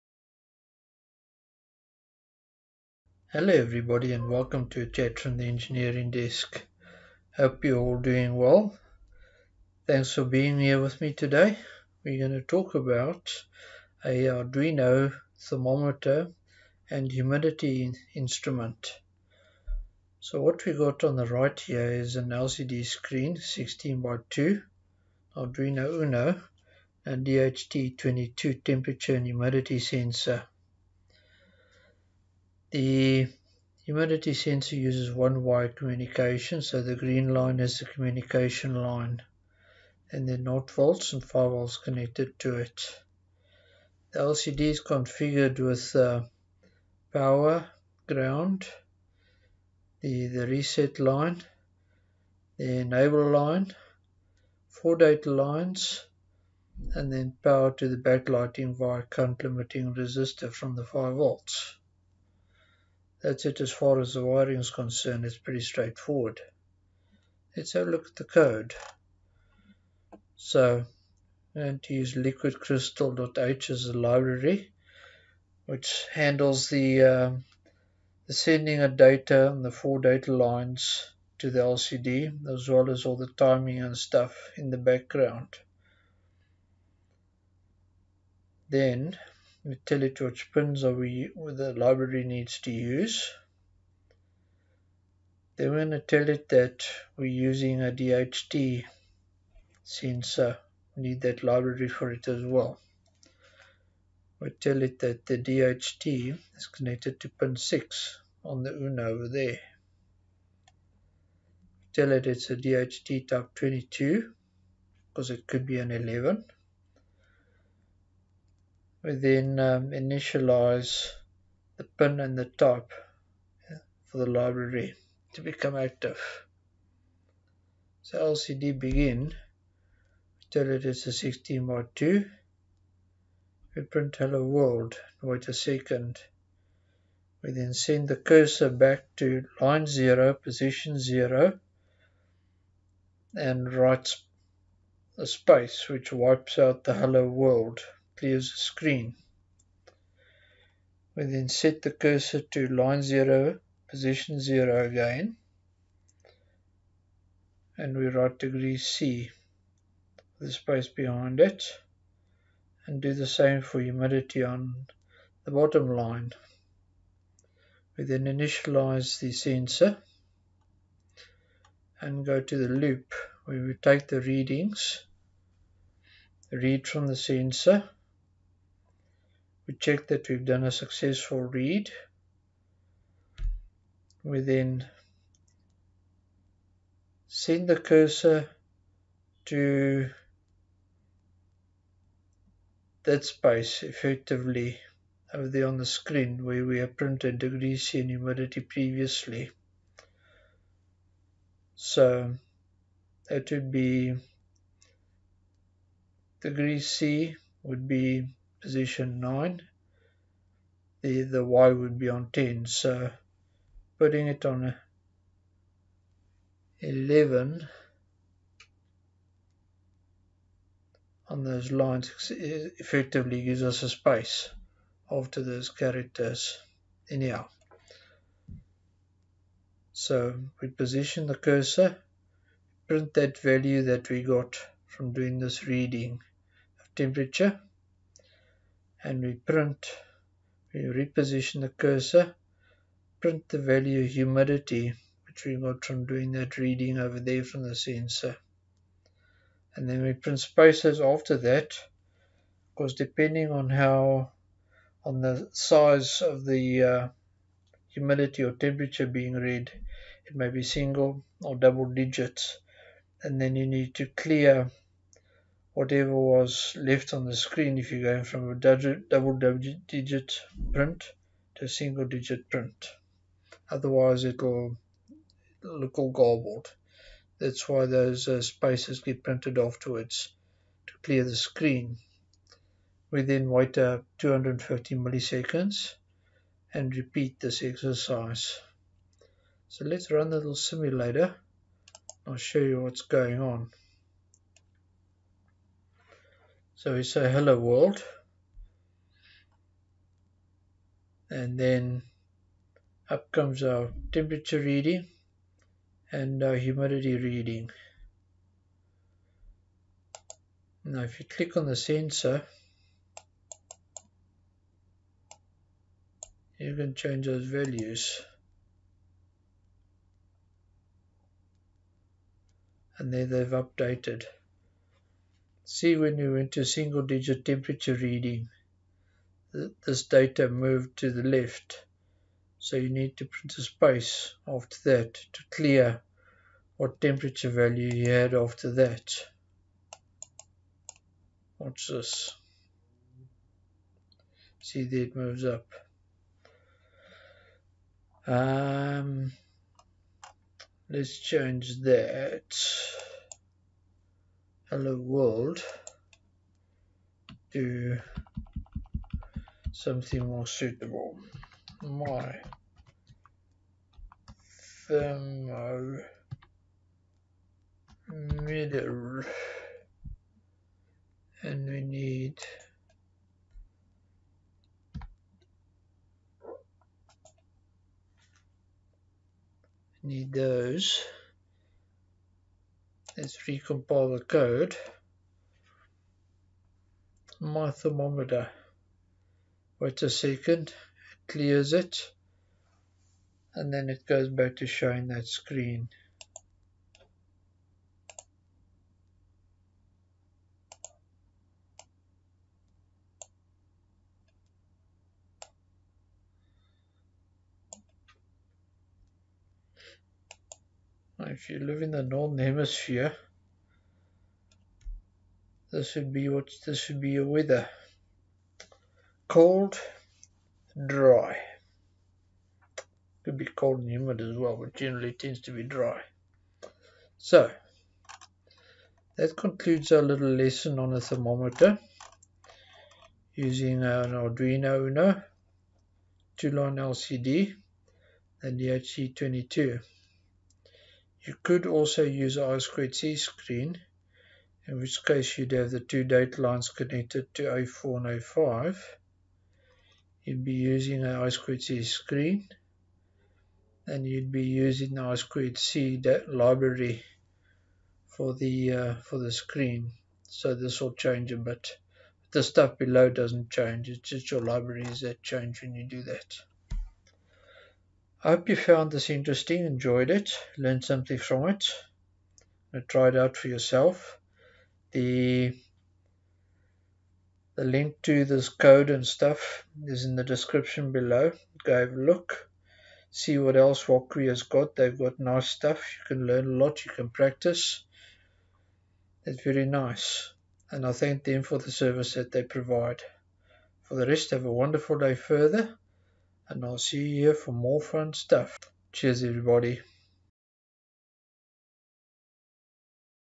Thermometer on Wokwi
In todays video I run a DHT22 temperature sensor on an Arduino Uno and display the values on a 16x2 lcd screen.

Code here:
The DHT22 sensor example

The code is a combination of snippets from these two sketches.